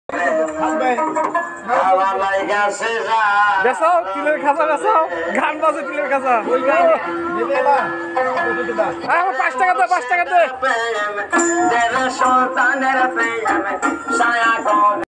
তিলের খাজা বুবু তিলের ।
অসোহায় লোকটি দিনের বেলায় রিকশার মধ্যে বক্স লাগিয়ে গান বাজি রিকশাচালায় আর সন্ধার মধ্যে তিলের খাজা